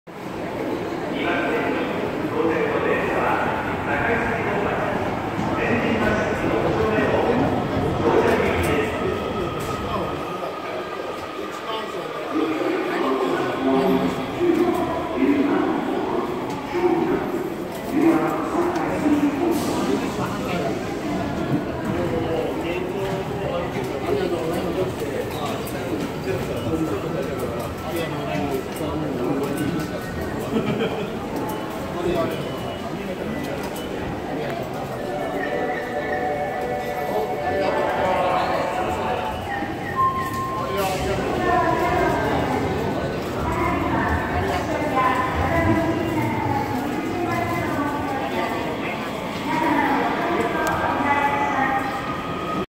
【改札口で挨拶してくれるOsaka Metoroの駅員さん】
【改札口で挨拶してくれる駅員さん】

Osaka Metoro《堺筋線》日本橋駅(中北改札口)にて撮影

撮影日 2019年3月7日(木) 20時19分頃